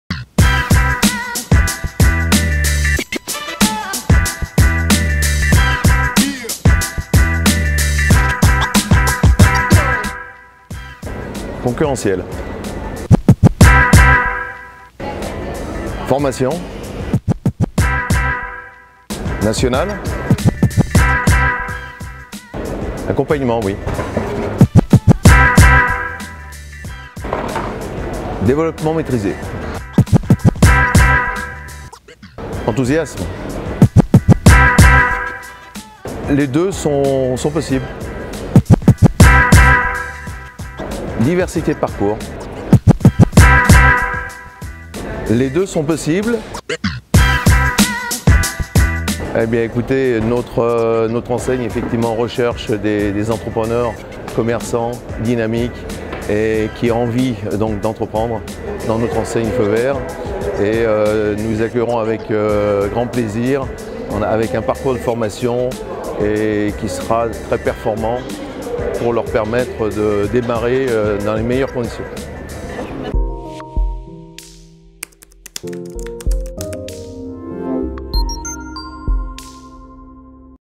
Interview Pile ou Face - Vincent Marion
Vincent Marion, responsable service expansion de l'enseigne Feu Vert, nous présente l'enseigne lors de Franchise Expo Paris.